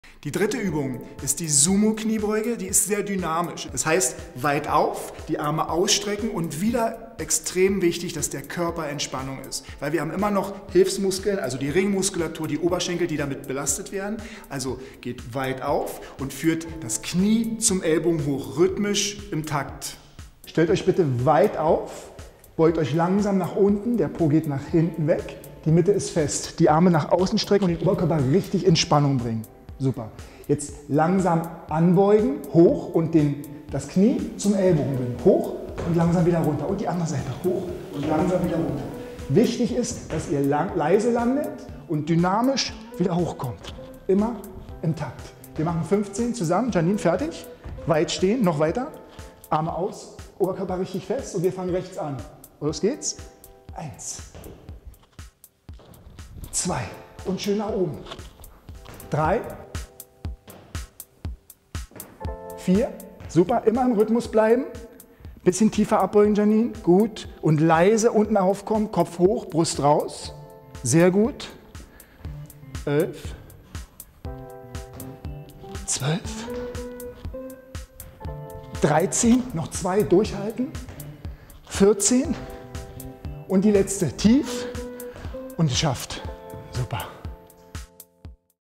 Das Hakle Kusszonen Workout! Übung 3
Das ultimative Workout für den Po! Mit Karsten Schellenberg und dem Playmate des Jahrtausends, Janine Habeck.

Regie: Lars Pape